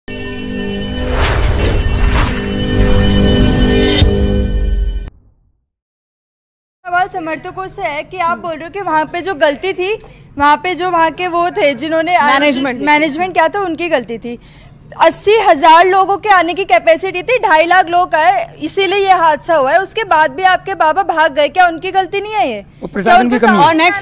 Why is Bhole Baba(Suraj Pal Jatav) not being held accountable? I Network18
Everyone knew the management was at fault. They allowed 2.5 lakh people to gather, far exceeding the permitted 80,000. But this despite this clear mistake, Baba is nowhere to be found. Why is he not being held accountable?
Jeeya
BAJMC2